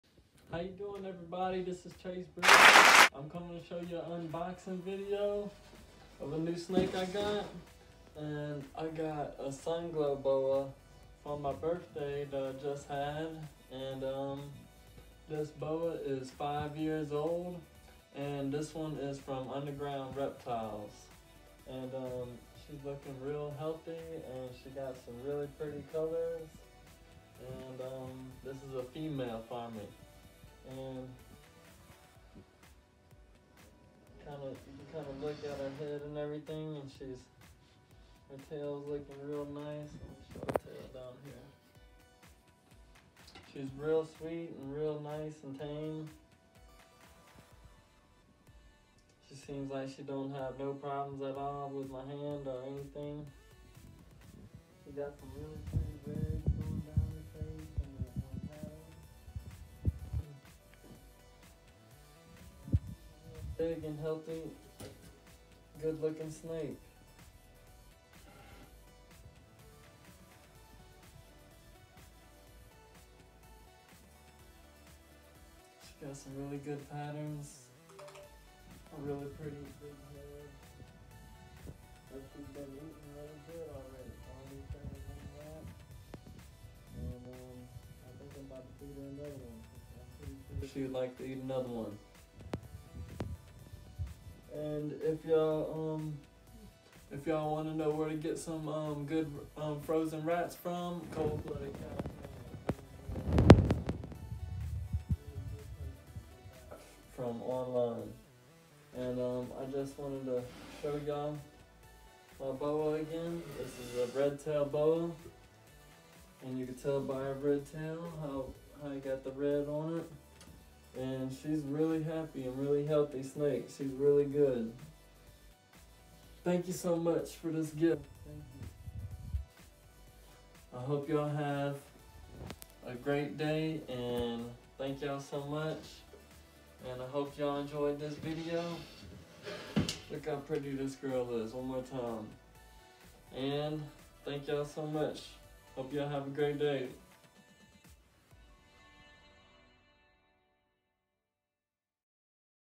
( SNAKE UNBOXING ) FROM ( UNDERGROUND REPTILES) MY BIRTHDAY GIFT FROM MY WIFE !!
Hey Everyone Hope You Are Having A Great Day Today It Is Getting Cold Here !!!

Sharing With You Today Is A Birthday Gift From My Wife Thank You So Much !!!
Female Sunglow Boa 5 Years Old From
Underground Reptiles !!

Editing Done By Haley Brunner !!!